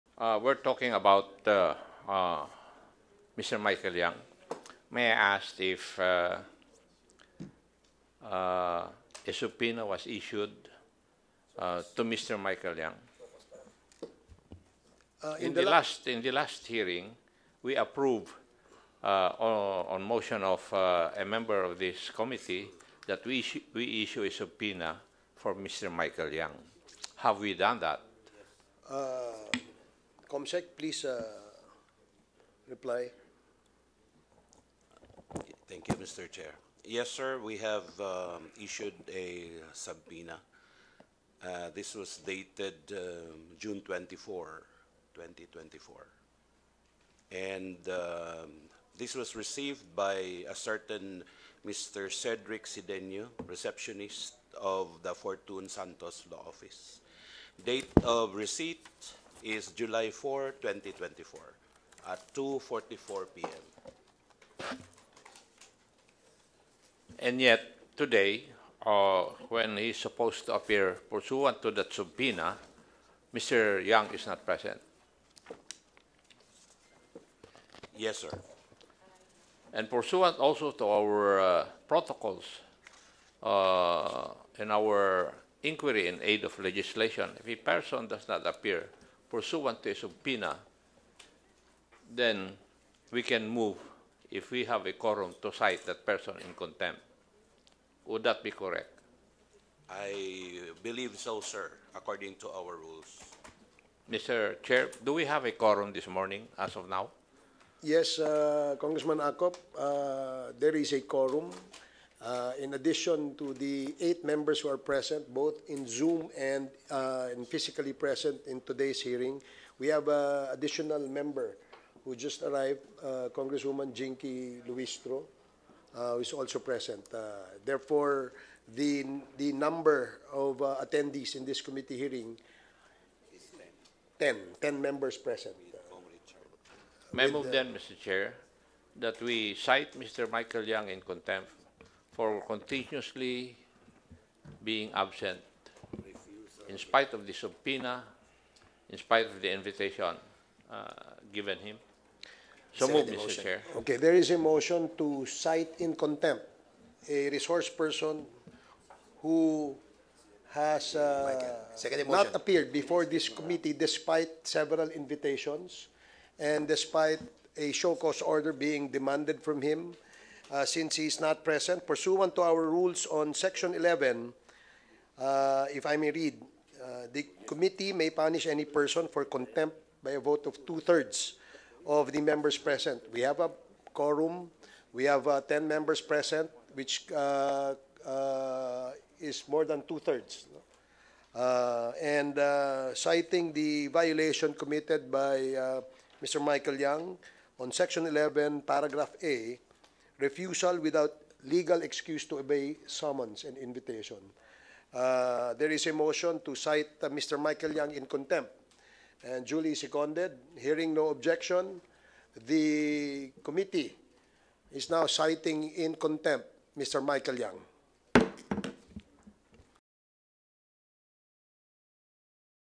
Michael Yang, na-cite in contempt ng House panel
Na-cite in contempt ng House Comittee on Dangerous Drugs si dating presidential economic adviser Michael Yang dahil sa hindi pagdalo sa mga pagdinig ng komite ukol sa illegal drugs.

Nitong Mayo pa nag-isyu ng show-cause order ang komite kay Yang pero hindi ito sumipot sa mga hearing.

Iniuugnay umano si Yang sa kumpanyang "Empire 999" na nagmamay-ari ng bodega kung saan may nahuling bilyon-bilyong halaga ng ilegal na droga. News5 | via Marianne Enriquez